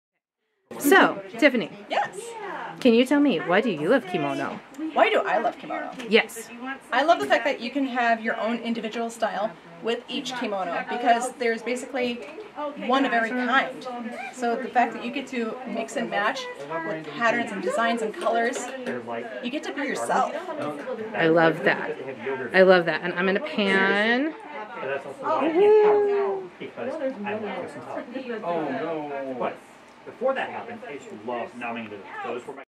Educational content! Why does Tangerine Tiffany Love Kimono?
We hope you enjoy this snippit from an impromptu educational moment had during our Kimono Day and Grand Opening celebration on 11/15/2021.  In this segment:  Why Tiffany loves kimono!  Share why you love kimono in the comments below!